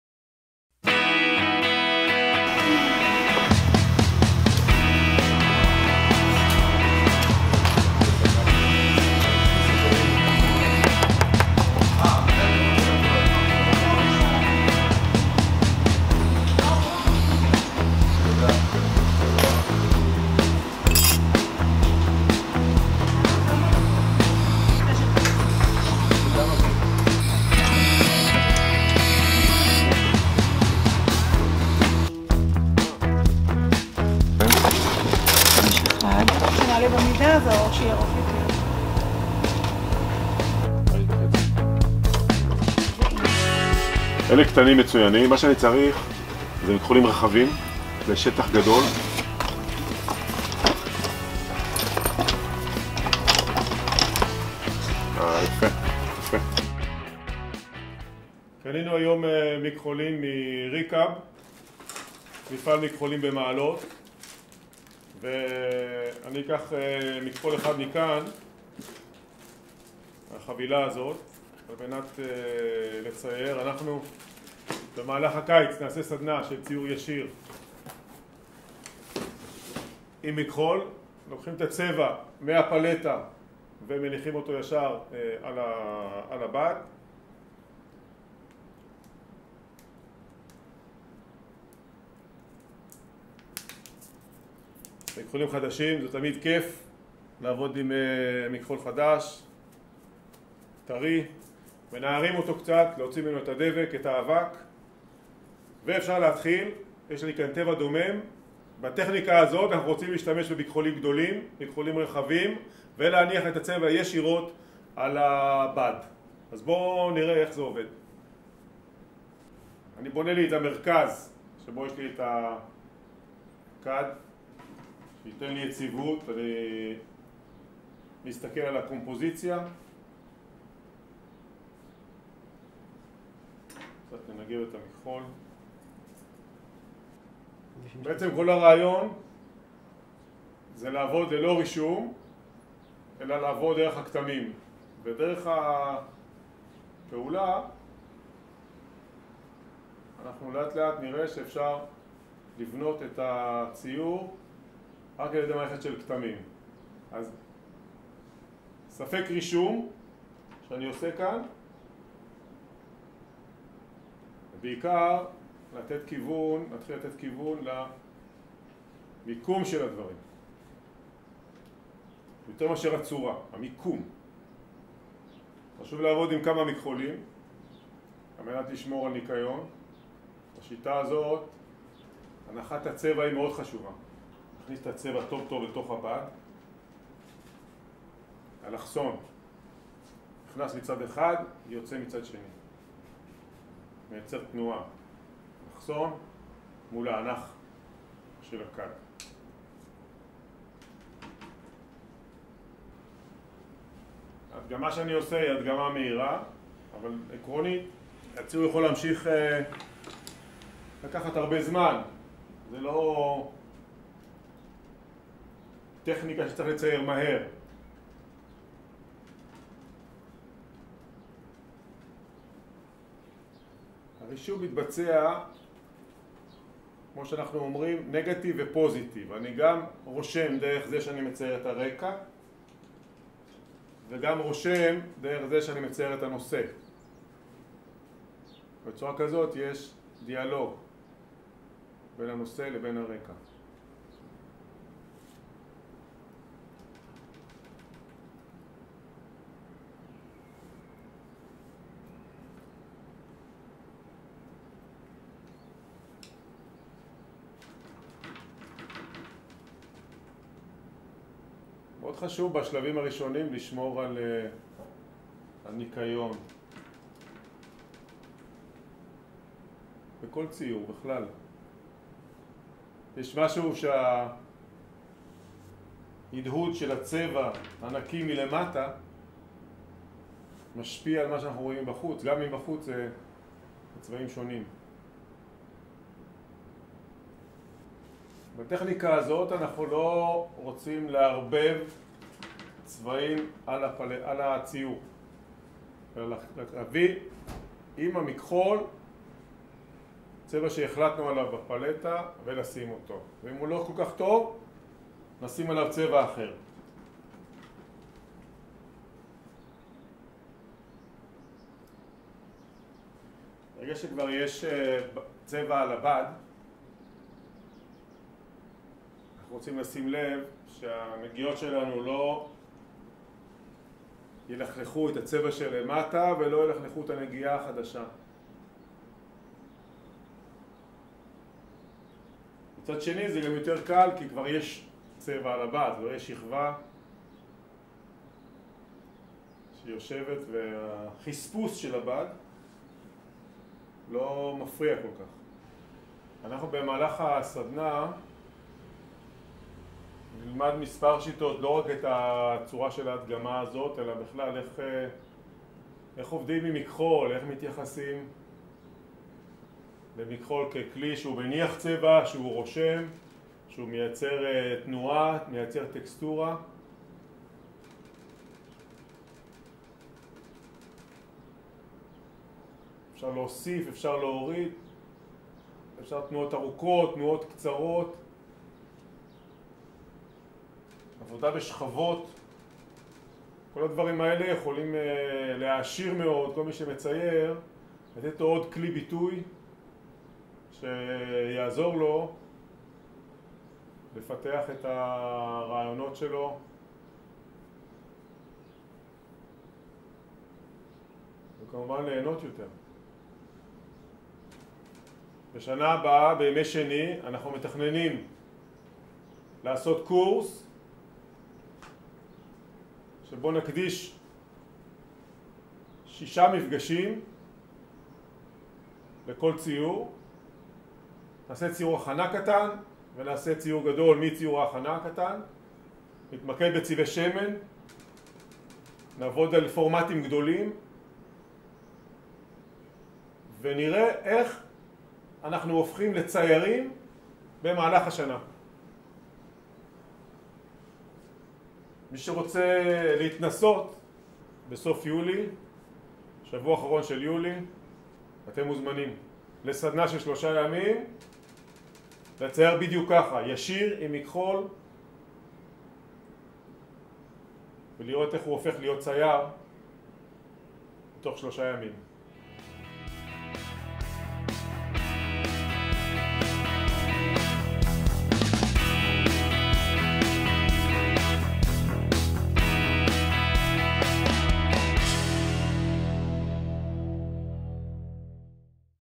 סדנת ציור בשמן עם סיור במפעל מכחולים
אמיר ניר בביקור במפעל מכחולים ובהדגמת ציור ישיר עם מכחול - שמן על בד